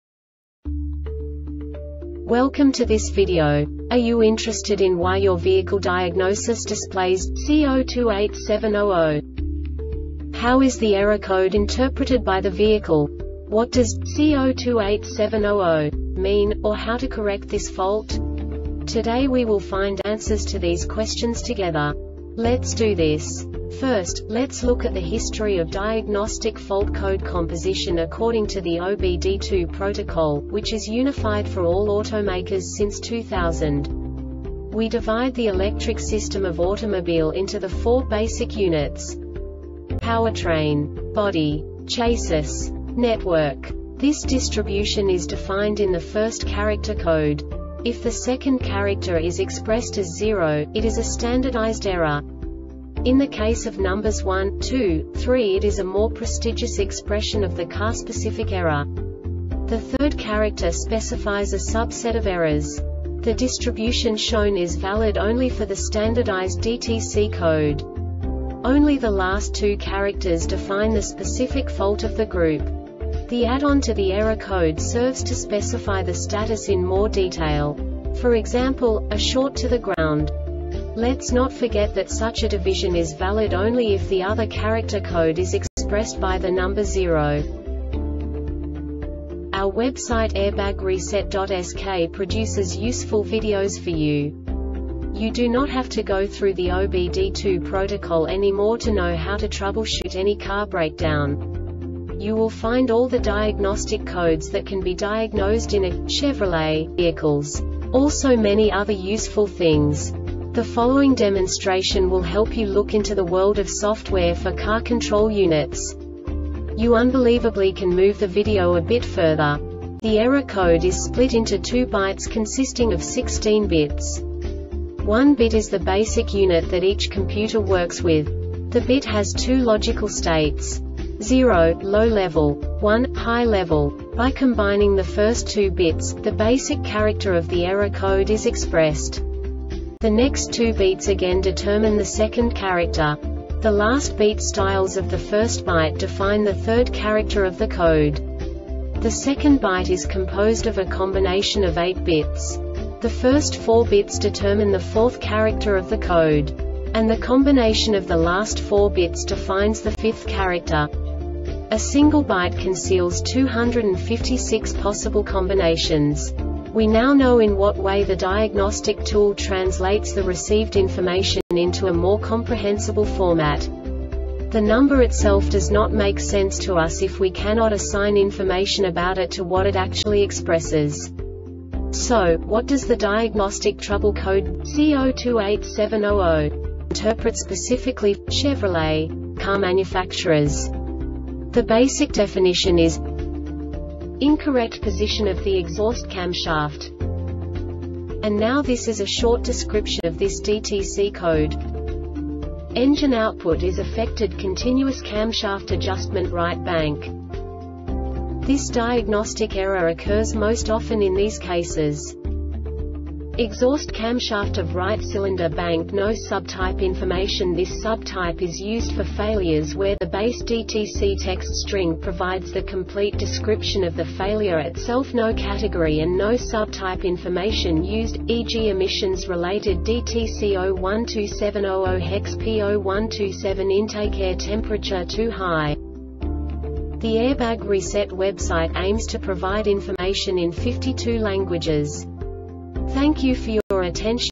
DTC Chevrolet C0287-00 Short Explanation
Contents:
0:21 Basic DTC analysis according to OBD2 protocol standard.
1:48 Insight into programming
2:58 A diagnostically understandable description of the Diagnostic Trouble Code
3:05 Basic error definition

Make:
Chevrolet
Code:
C0287 00
Definition:
Incorrect Position of the Exhaust Camshaft
Description:
Engine output is affected.Continuous camshaft adjustment (right bank).
Cause:
Exhaust camshaft of right cylinder bank.